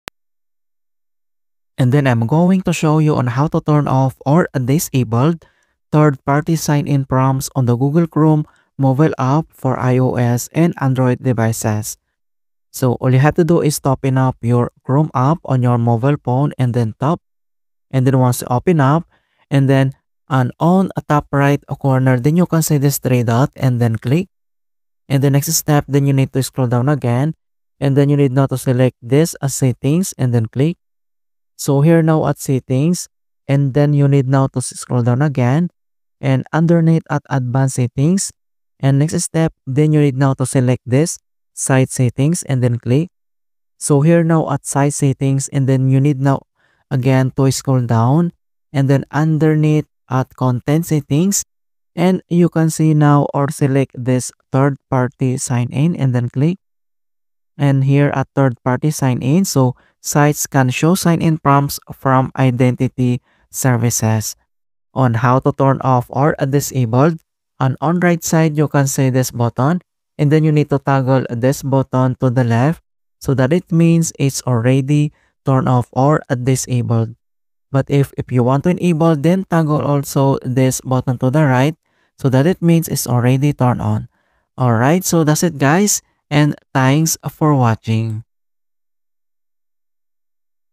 How To Turn Off Third Party Sign-in Prompts on Chrome App
This comprehensive guide will show you how to effectively turn off or disable third-party sign-in prompts within the Google Chrome mobile app. If you're looking to stop constant pop-ups from websites asking you to "Sign in with Google," "Sign in with Apple," or other services, this tutorial is for you. Learn how to manage site settings, improve your Browse privacy, and reduce digital distractions on your Android or iOS device.

Manage third-party sign-in prompts
To select your third-party sign-in prompt preferences:
1. On your Android device, open Chrome Chrome.
2. At the top right, tap More  and then Settings.
3. Under Advanced, tap Site settings and then Third-party sign-in.
4. Select the setting you like: Sites can show sign-in prompts from identity services. Block sign-in prompts from identity services.